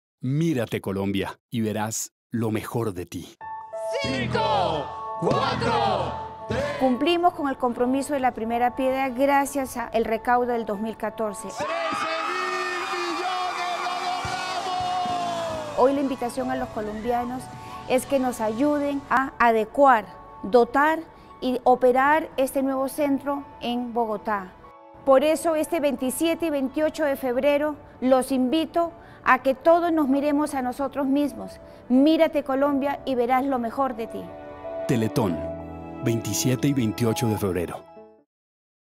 Emilia Unidad Nacional
La presidenta de Teletón Colombia invita a todos los colombianos a que nos miremos diferente MirateColombia y verás lo mejor de ti.